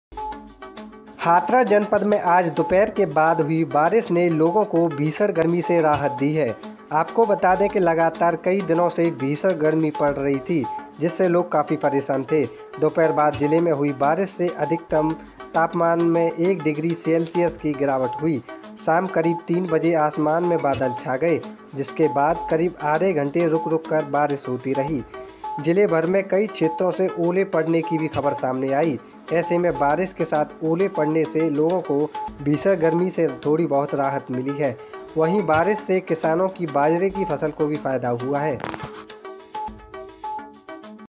मुरसान : बारिश के साथ ओलों ने भी दस्तक
मुरसान : हाथरस जनपद में आज  दोपहर के बाद  हुई बारिश ने लोगो को भीषण गर्मी से राहत  दी है । आपको बता दे कि  लगातार कई  दिनो से भीषण गर्मी पड़ने से लोग काफी परेशान थे । दोपहर बाद जिले में  हुई  बारिश से  अधिकतम तापमान में 1 डिग्री सेल्सियस की गिरवाट हुई है । शाम करीब तीन बजे आसमान में बादल छा गए। जिसके बाद करीब आधे घंटे रुक-रुक कर बारिश होती रही - जिले भर में कई क्षेत्रों से ओले पड़ने की भी खबर सामने आई । ऐसे में बारिश  के साथ ओले पड़ने से  लोगो को  भीषण गर्मी से थोड़ी बहुत राहत मिली है । वही बारिश से किसानों की  बाजरे की फसल को भी फायदा हुआ है ।